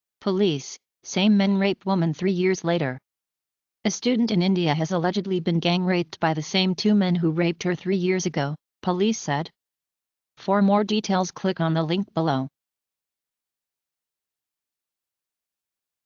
Police: Same men rape woman 3 years later | By : CNN
A student in India has allegedly been gang-raped by the same two men who raped her three years ago, police said.